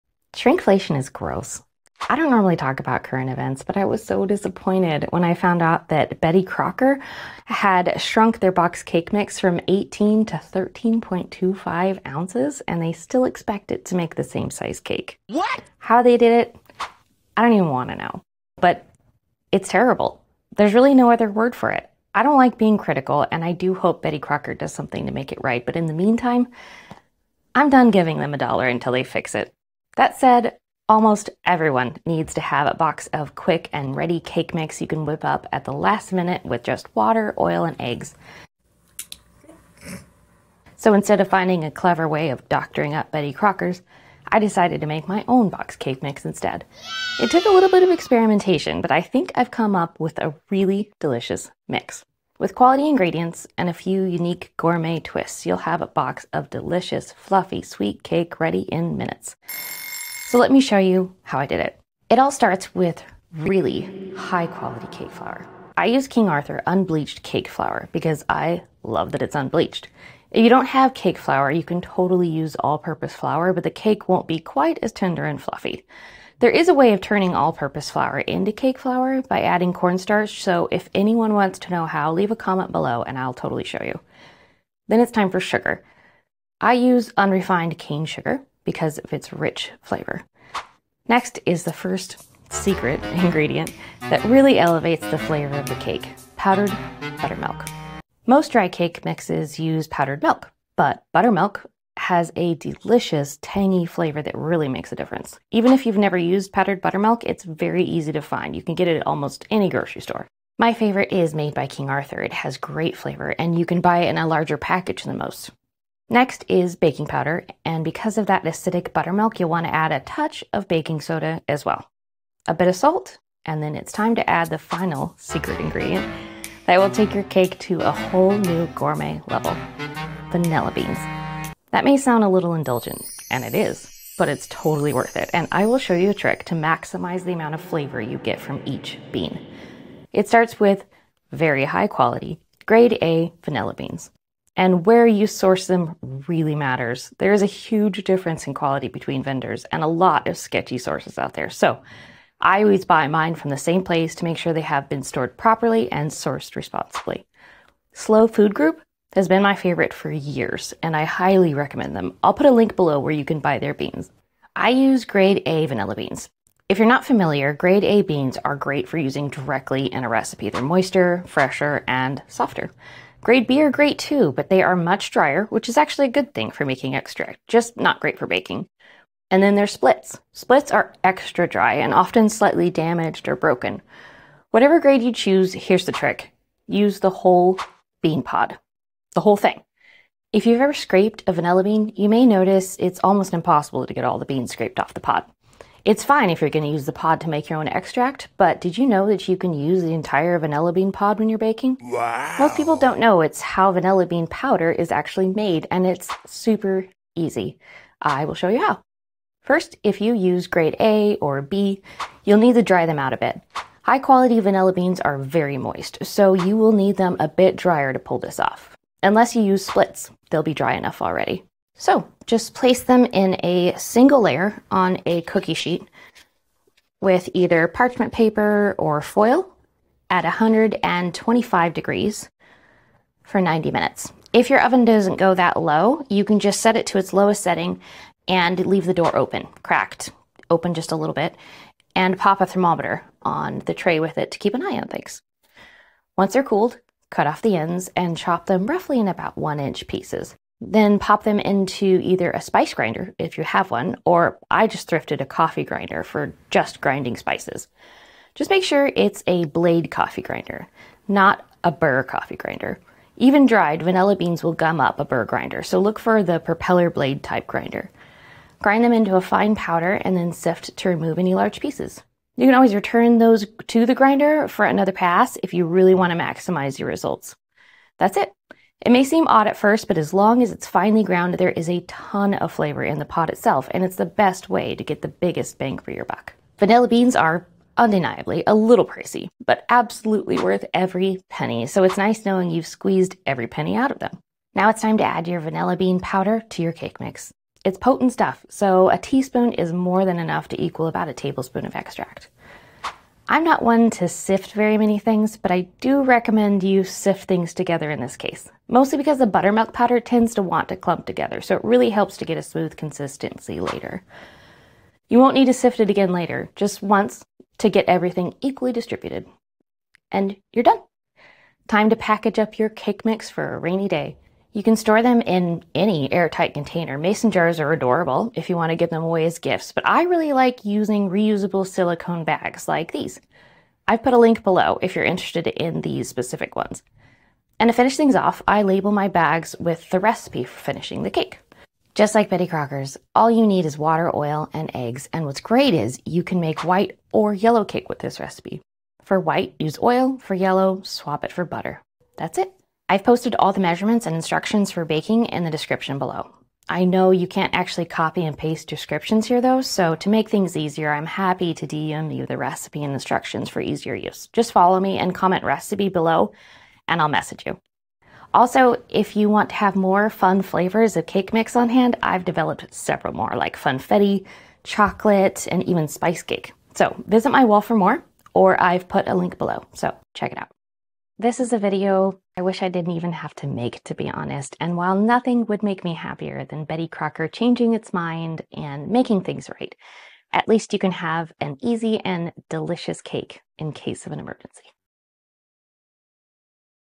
Homemade Delicious Cake Mix: No More Shrinkflation!
Fed up with shrinkflation and smaller portions from Betty Crocker? Learn how to make your own high-quality, gourmet cake mix at home with this easy recipe. I'll show you the secret ingredients and techniques to elevate your cake mix game, including using real vanilla bean powder for maximum flavor. Say goodbye to disappointing boxed mixes and hello to fluffy, delicious homemade cakes every time!

BASIC HOMEMADE BOXED CAKE MIX RECIPE
2 cups cake flour (250 g)
11/2 cups sugar (300 g)
1/2 cup buttermilk powder (80 g)
1 Tablespoon Baking Powder
1/2 teaspoon baking soda
1 teaspoon salt
1 teaspoon Vanilla Bean Powder
FLAVOR OPTIONS:
Lemon- add 2 Tablespoons lemon zest, dried (from 2 large or three small lemons)
Funfetti- add 1/2 cup rainbow sprinkles
• Chocolate- add 3/4 cup cocoa powder
Spice- add 1 heaping Tablespoon Pumpkin Spice
Ready to bake? Lemon and Funfetti stick to the basic oil, water, eggs, but chocolate and spice need an extra something special, so see th recipes below:
Lemon Cake
1 cup water
1/2 cup oil
3 eggs
Funfetti Cake
1 cup water
1/2 cup oil
3 eggs
Chocolate Cake
1 cup water
1/2 cup oil
1/2 cup melted butter
3 eggs
Spice Cake
1 cup apple sauce, unsweetened
1/4 cup water
2 Tablespoons Molasses
1/2 cup oil
3 eggs

BAKING INSTRUCTIONS
Bake 350° (375° for cupcakes)
13"x9" sheet cake: 26-30 minutes
Two 8" rounds: 26-30 minutes
Two 9" rounds : 25-29 minutes
Ring Mold : 36-40 minutes
Cupcakes (makes 22) : 14-18 minutes